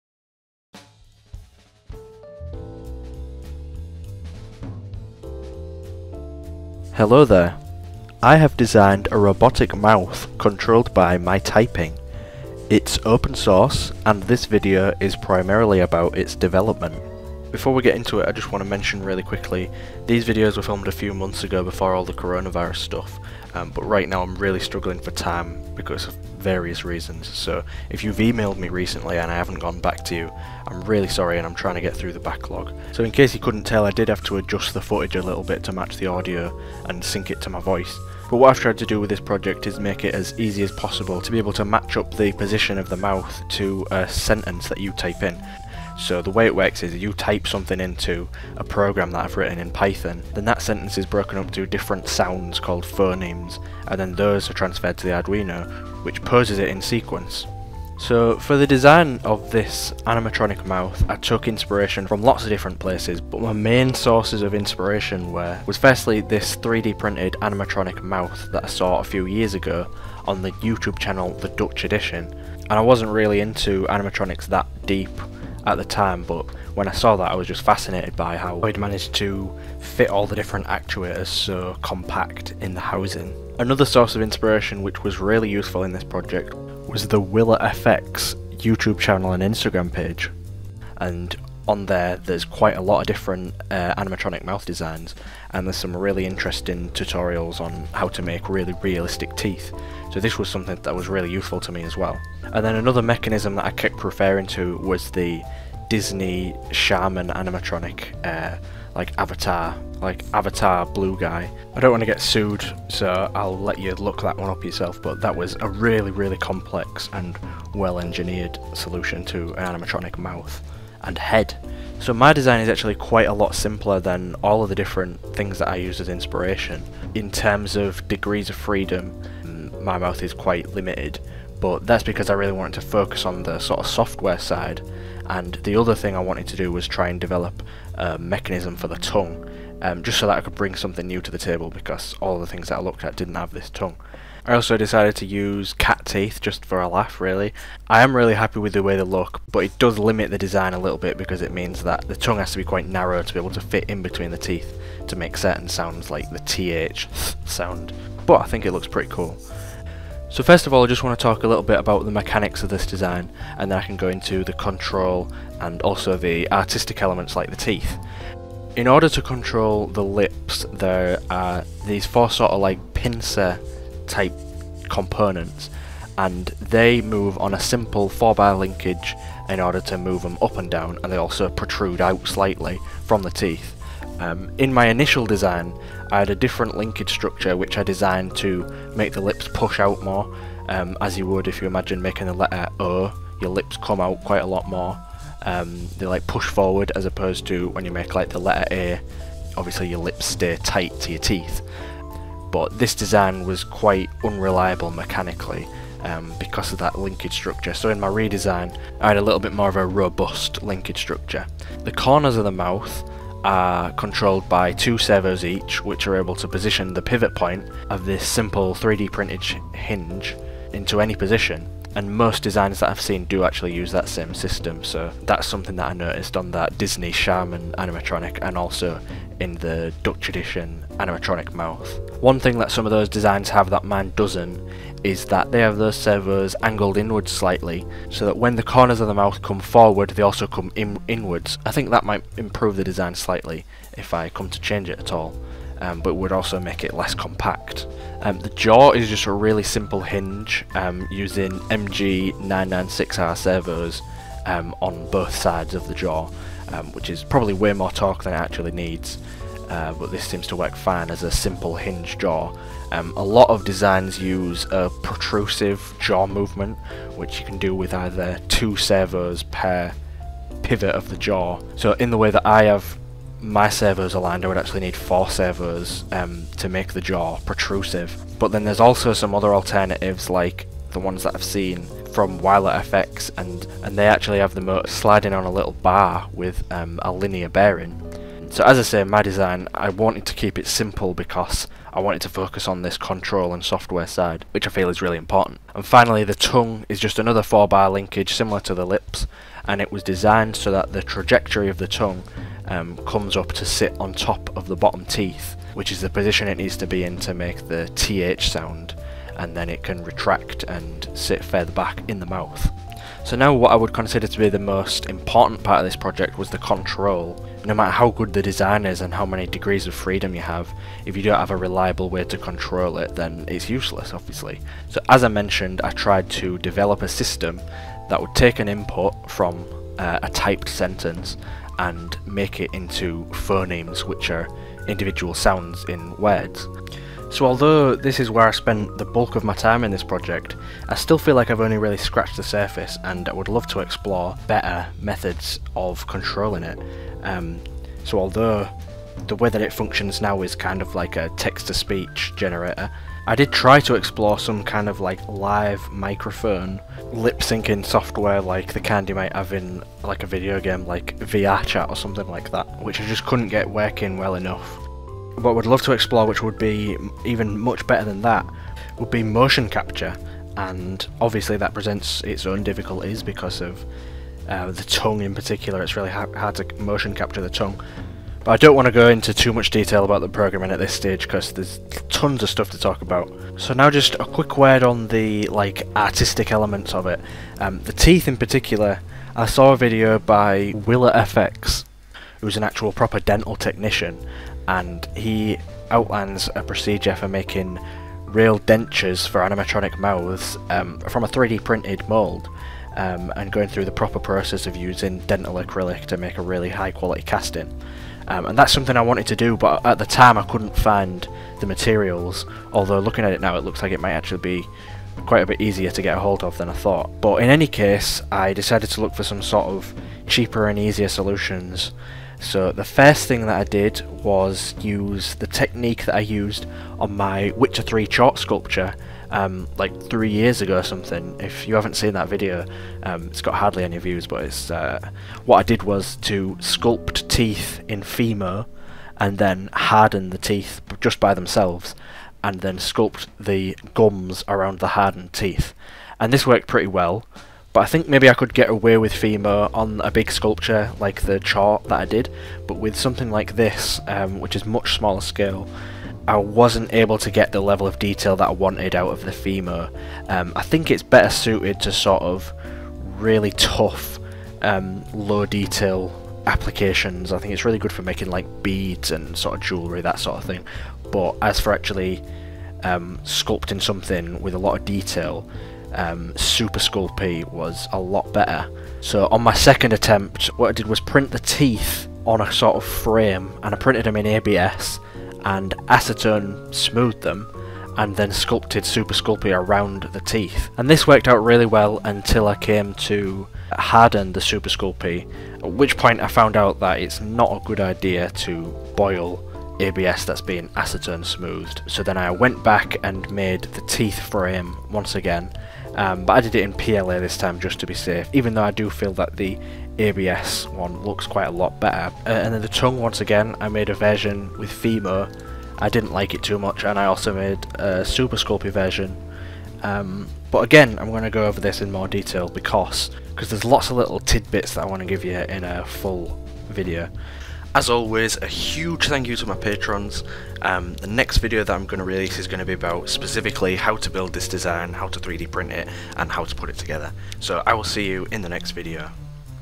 Developing an Animatronic Mouth Mechanism
Here's the animatronic mouth mechanism I showcased in my recent video. In this video I talk briefly about the development of the mechanism, programming and artistic elements, and I'm going to be taking you through how to build one yourself!

🔌 Power Supply
🔥 Soldering Station